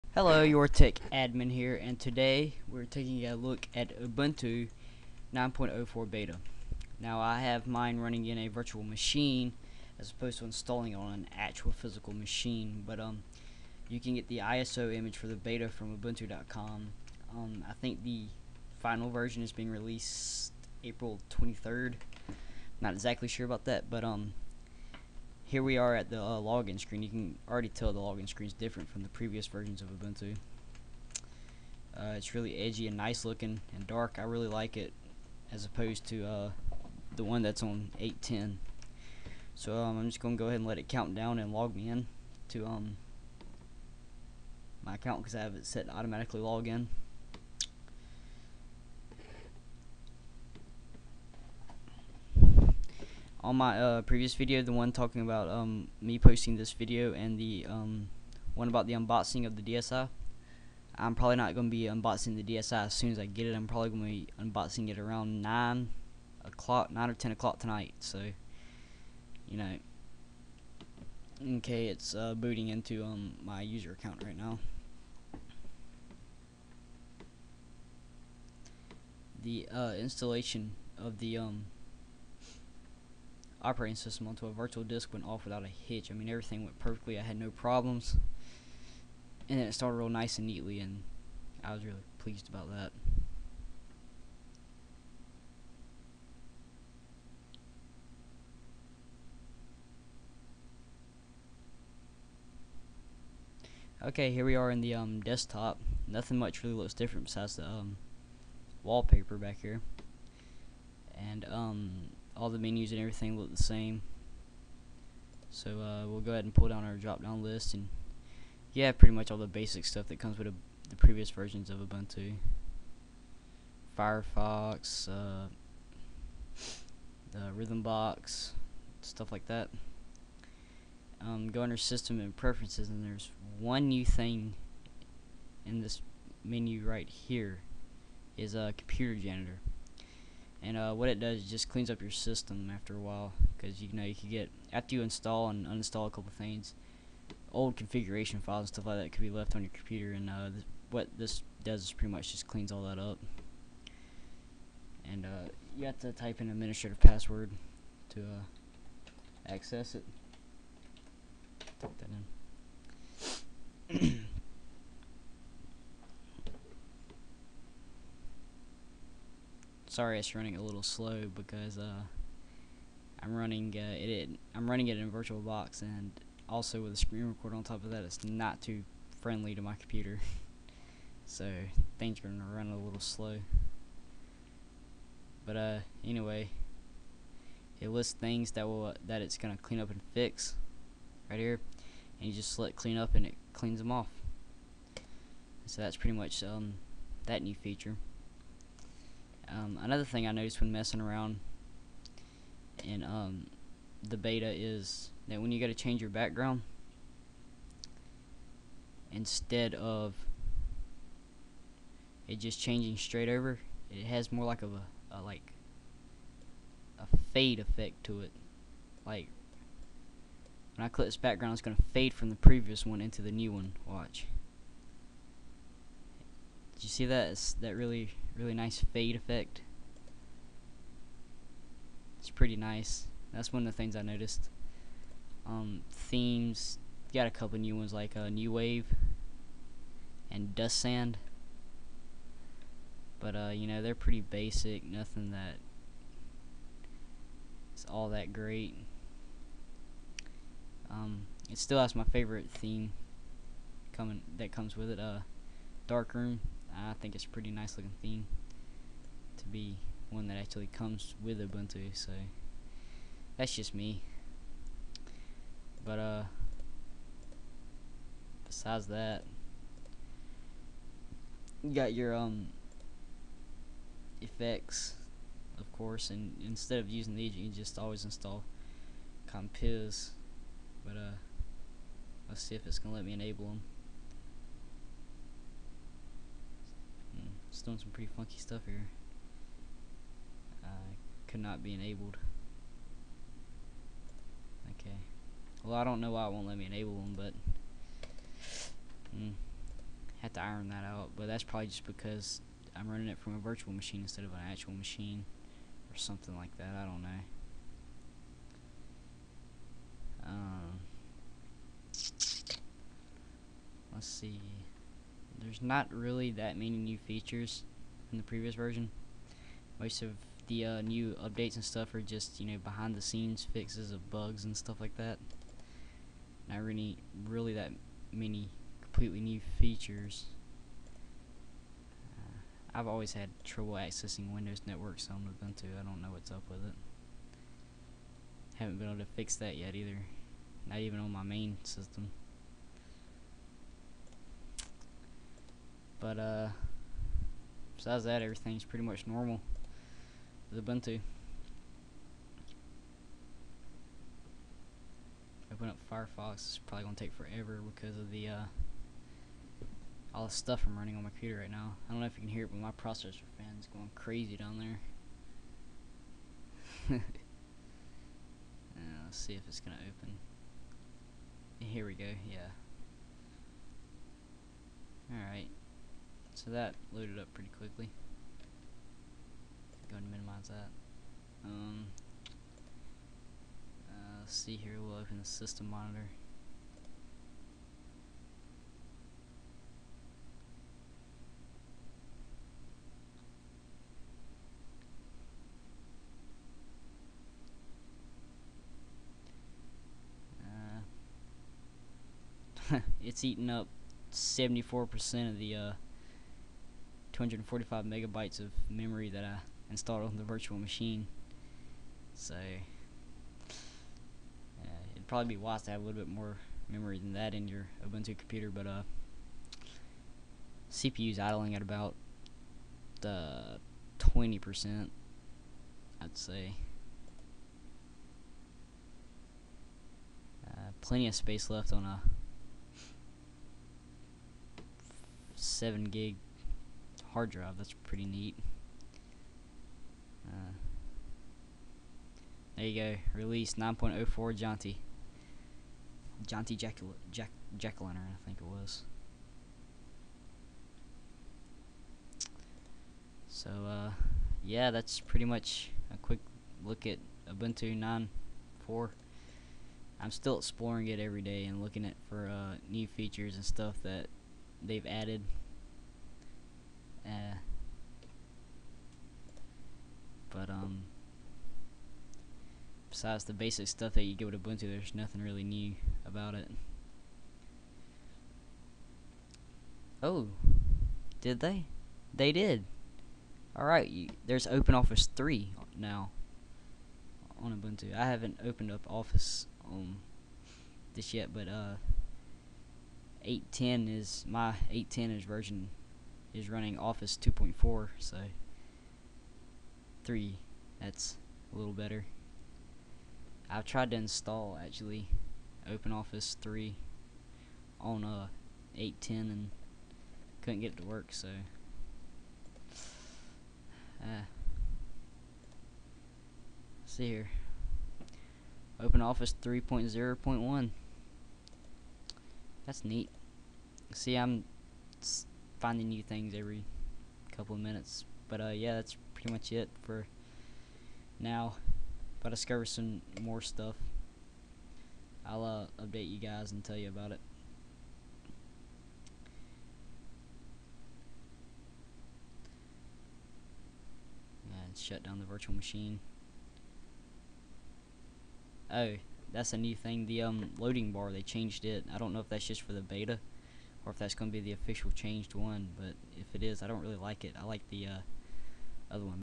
Ubuntu 9.04 Beta Overview
I installed Ubuntu 9.04 beta on a virtual machine. Here are my thoughts.

Note: The DSi unboxing will be later tonight around 9:00 PM EST